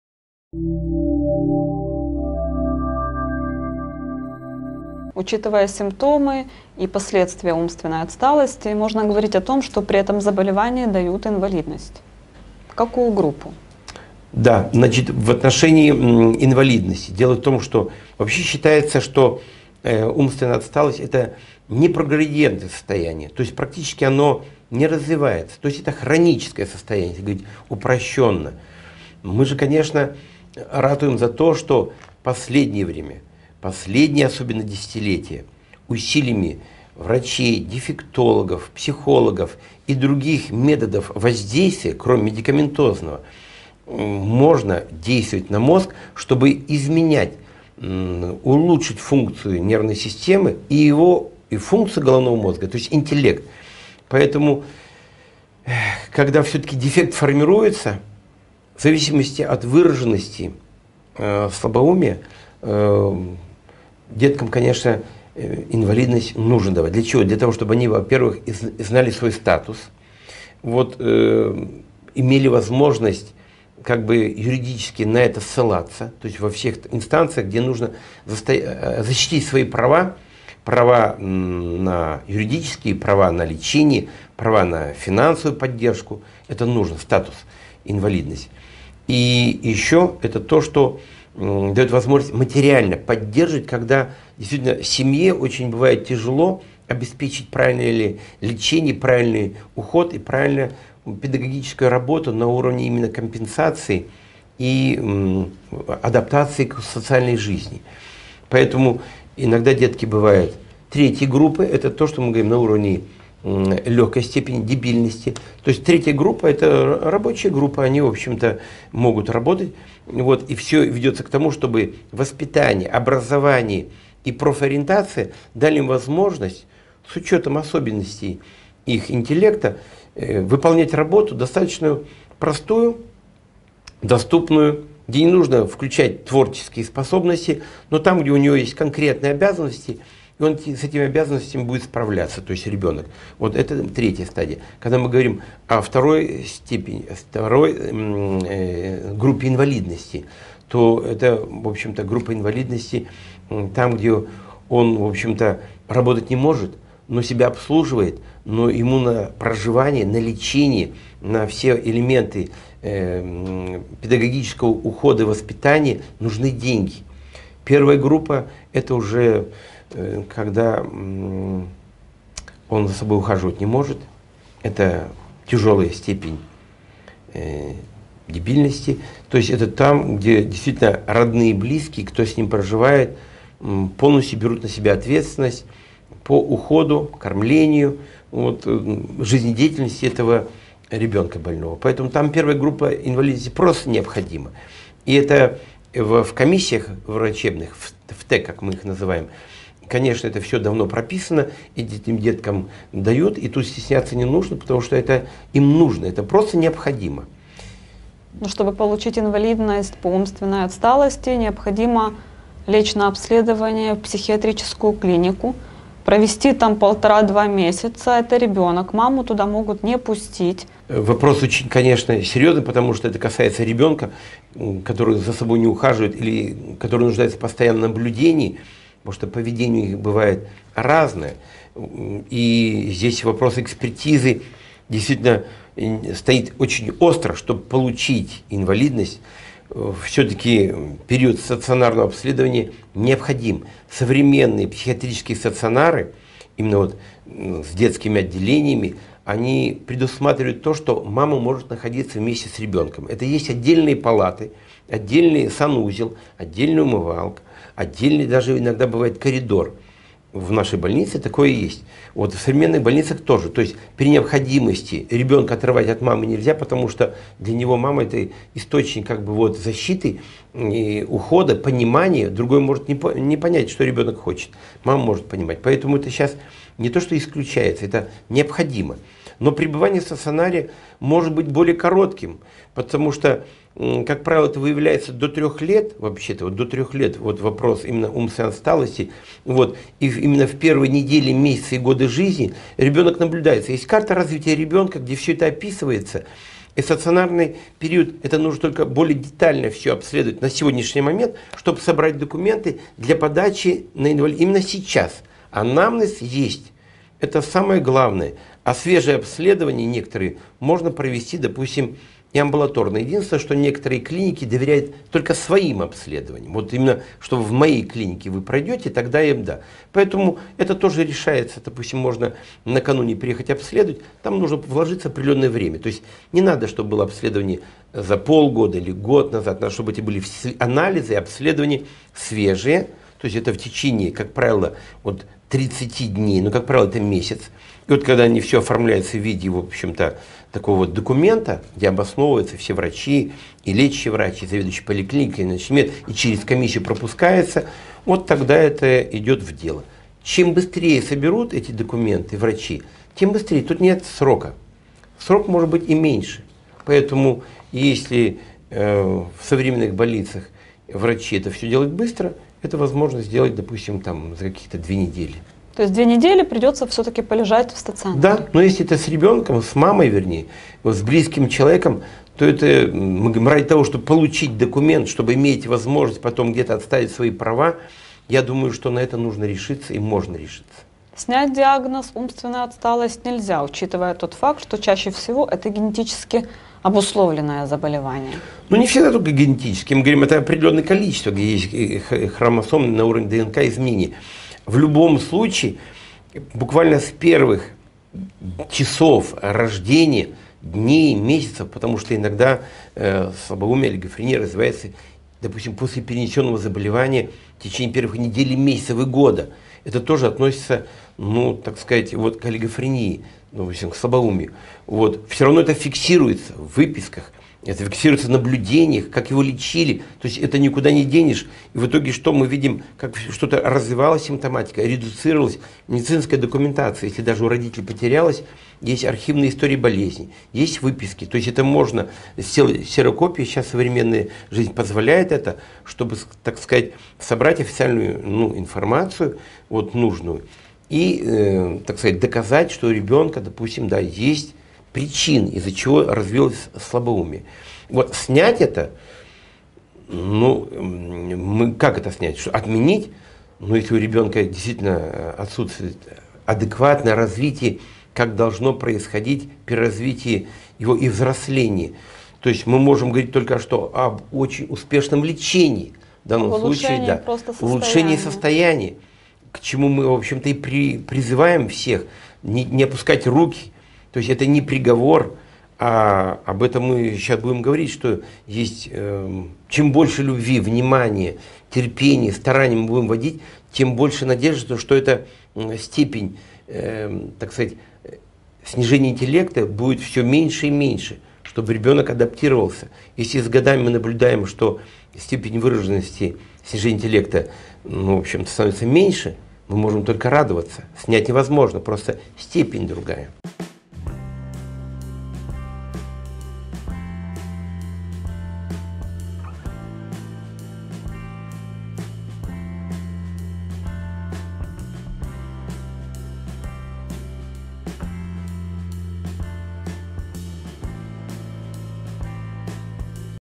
Как получить инвалидность при умственной отсталости (Олигофрении)
Оглавление:
1. 0:05 Инвалидность при умственной отсталости.
2. 3:28 Как получить инвалидность при умственной отсталости.
3. 6:21 Как снять диагноз умственная отсталость.

Ведущий:Ирина Лисицына
Врач-психотерапевт высшей категории, кандидат медицинских наук: Галущак Александр Васильевич

Информация справочная. Необходимо проконсультироваться со специалистом

Инвалидность дают при умственной отсталости?

Правда ли, что для получения инвалидности необходимо пройти обследование в психиатрической клинике? При этом ребенку придется провести в стационаре без мамы порядка 1,5-2 месяцев?

Как снять диагноз умственная отсталость? И можно ли это в целом?